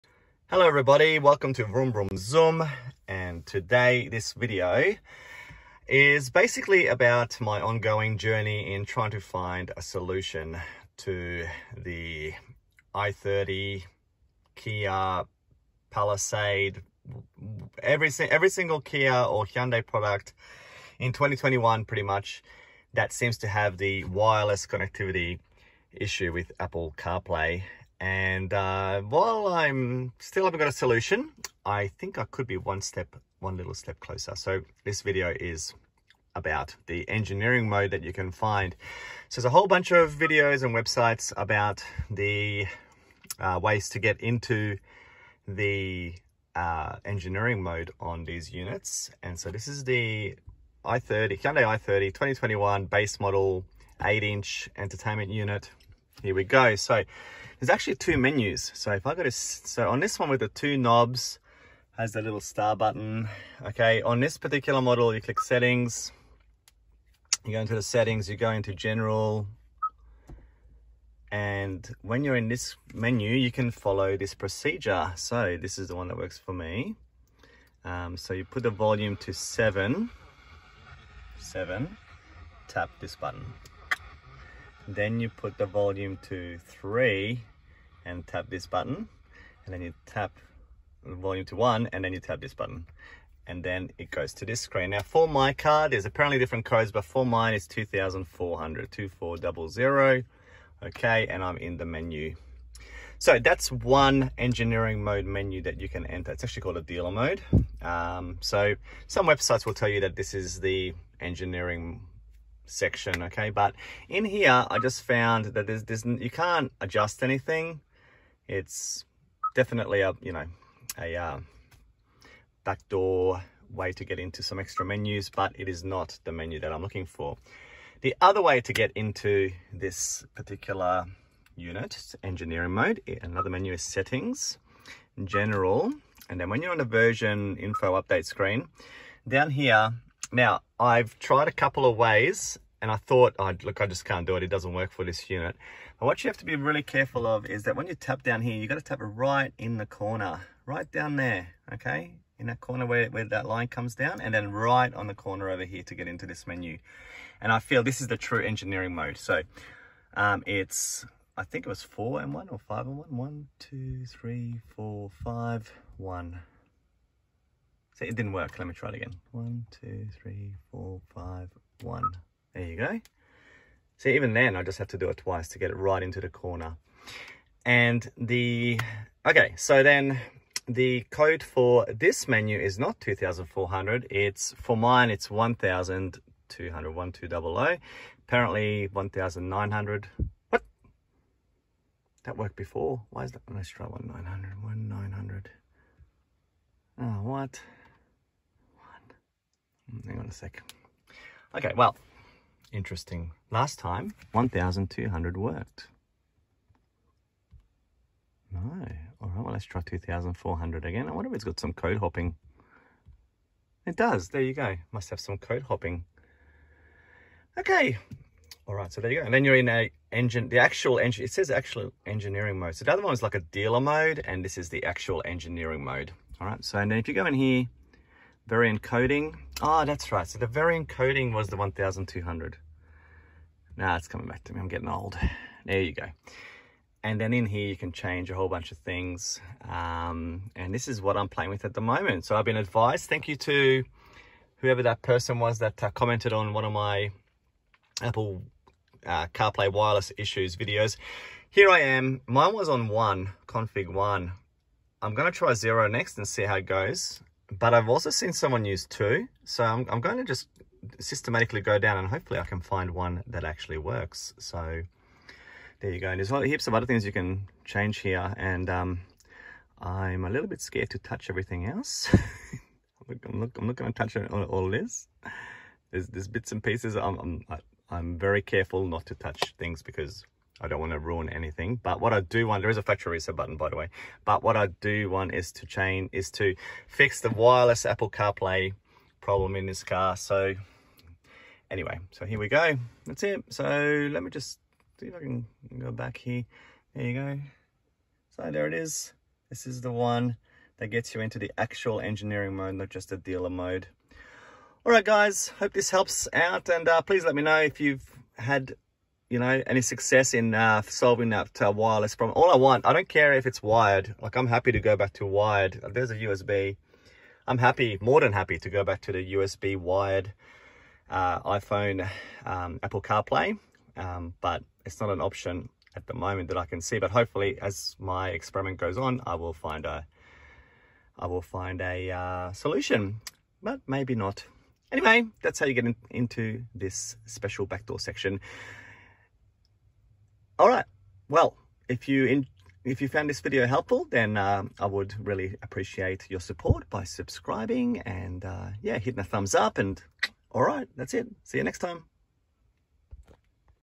Hyundai i30 PD 2020-2021 Engineering Mode and Dealer Mode Access!
In my search for solving the Apple CarPlay Wireless Connectivity issue that has plagued my brand new Hyundai i30 (base model here in Australia), an amazing viewer, Sabri Simsek, highlighted that there is an engineering mode on these systems! Anyway, after a bit more further researching, and Sabri pointing me in the right direction, I was able to access the elusive ENGINEERING MODE!!!

I will now start to play with the cpu config (through variant config) settings to see if I can solve the problem, or at least improve it somewhat. Ultimately wired CarPlay would be amazing.